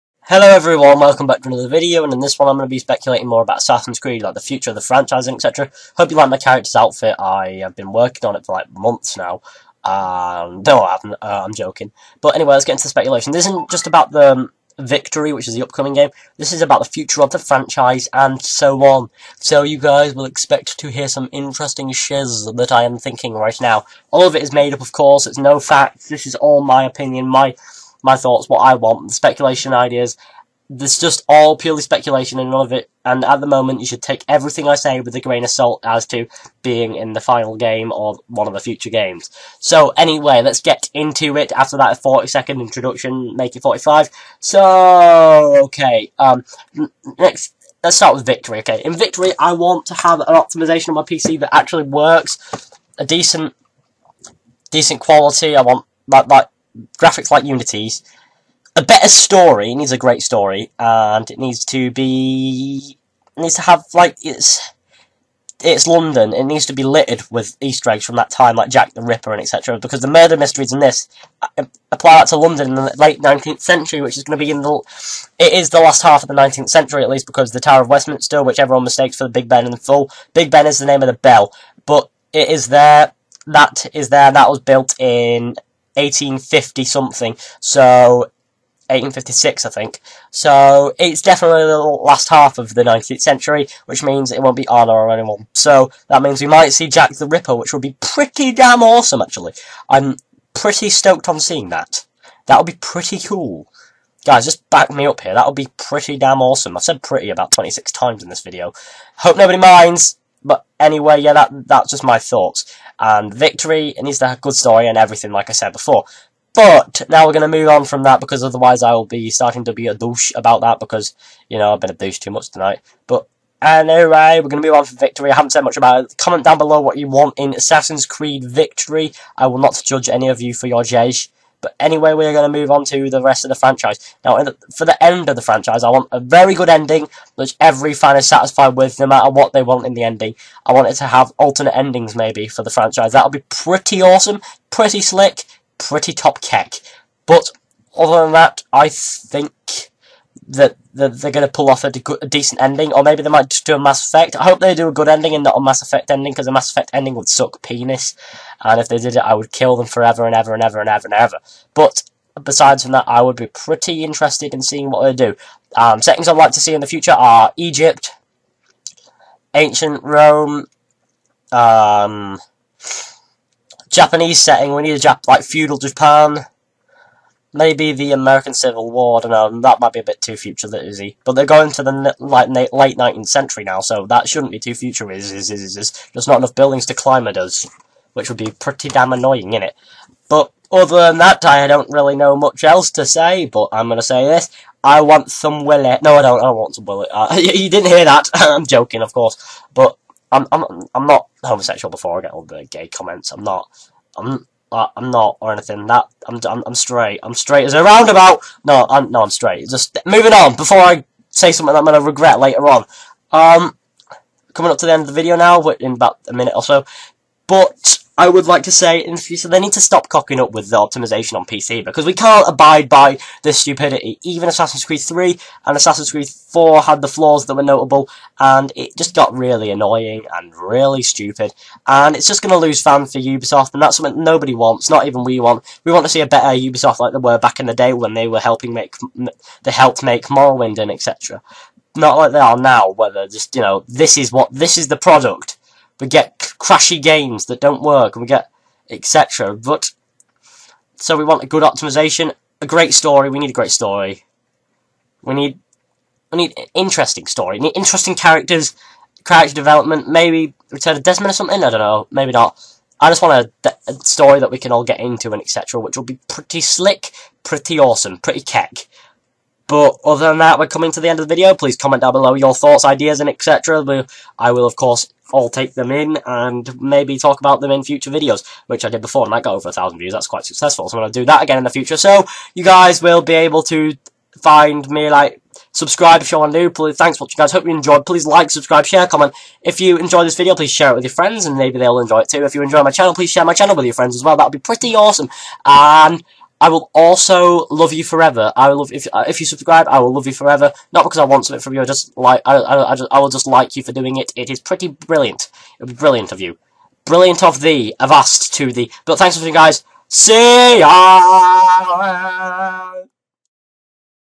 Assassin's Creed SPECULATION - Future Of The Franchise/Ideas For Victory
So, in this video, we speculate, not just Assassin's Creed Victory, but the future of the Assassin's Creed franchise beyond that too!

So, in Victory, I want a decent PC optimisation, a good story at least, a likeable protagonist and likeable side characters, a load of easter-eggs and a lot of depth, that would make me very happy!

For the rest of the franchise, I'd like to see Egypt and Ancient Rome which would be absolutely brilliant!